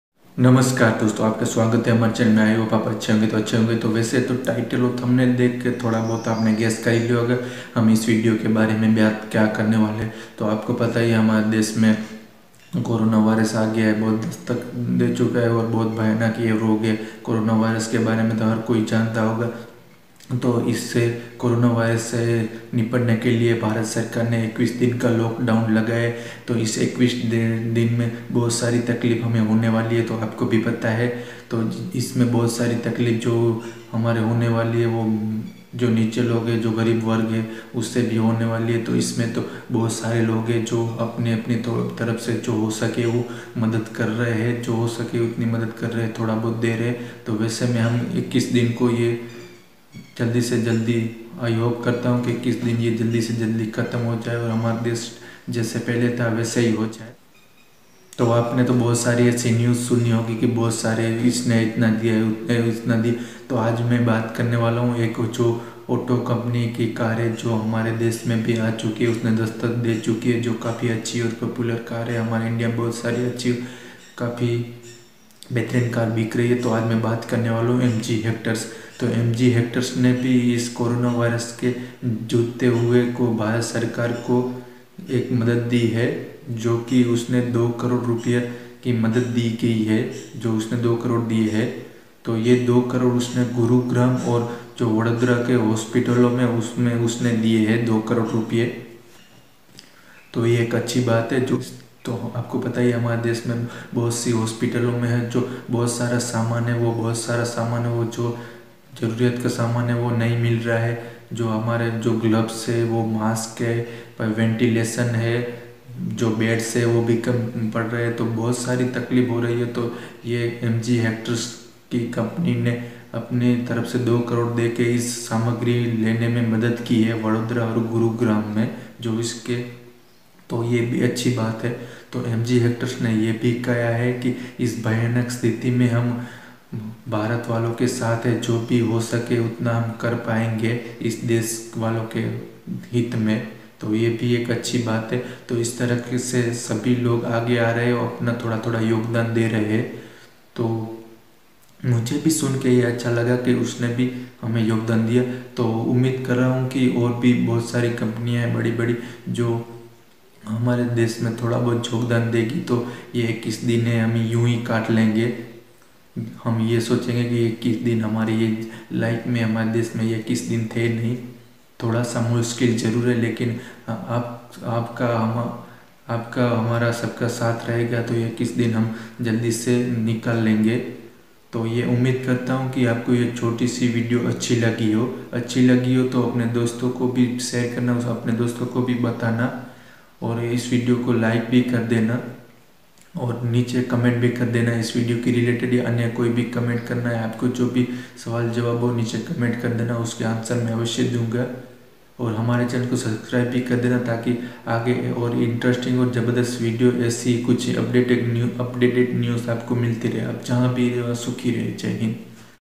Coronavirus के चलते MG HECTOR जो काम किया वो काम सभी company को करना चाहिए
namskar dosto

dosto is video mene apko MG hector ne Corona virus ke Chalte 2 crore diyehe

support